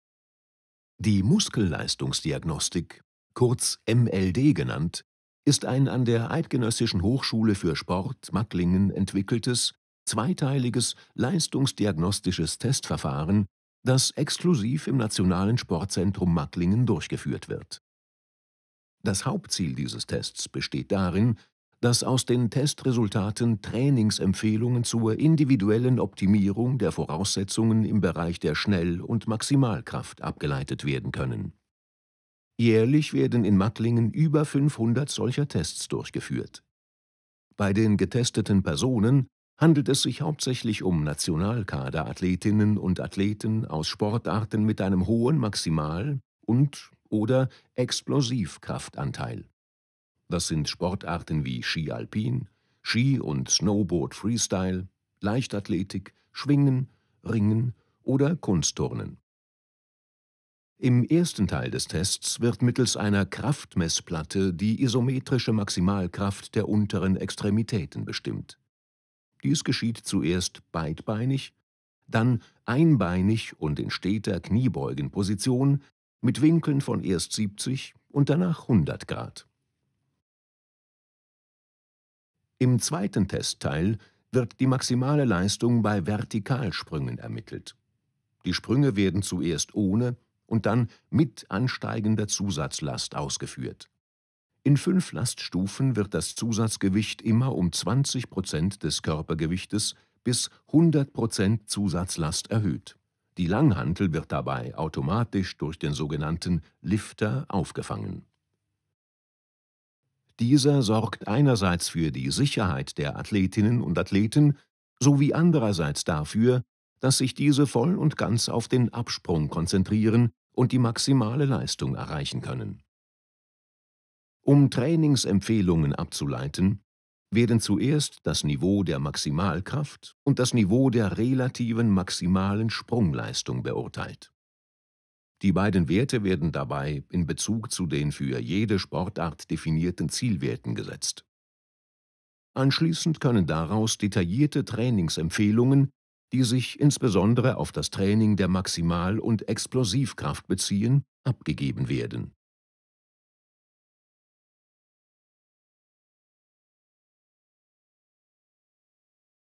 Kraftlabor / Laboratoire de force
Die Muskelleistungsdiagnostik (MLD) ist ein zweiteiliges leistungsdiagnostisches Verfahren, das an der EHSM entwickelt wurde und exklusiv im Nationalen Sportzentrum Magglingen durchgeführt wird. Die MLD wird jährlich bei über 500 Nationalkaderathletinnen und -athleten aus Sportarten wie Kunstturnen, Leichtathletik oder Ski Alpin angewandt.

Le diagnostic de performance musculaire est un diagnostic de la performance en deux parties, développé à la HEFSM et réalisé exclusivement au Centre sportif national de Macolin. Ce diagnostic est effectué chaque année auprès de 500 athlètes de cadres nationaux issus de sports tels que la gymnastique artistique, l'athlétisme ou le ski alpin.

La diagnostica delle prestazioni muscolari è uno strumento diagnostico costituito da due parti, sviluppato dalla SUFSM ed eseguito esclusivamente presso il Centro sportivo nazionale di Macolin. Ogni anno ne beneficiano più di 500 atlete ed atleti dei quadri nazionali delle discipline sportive come la ginnastica artistica, l'atletica leggera e lo sci alpino.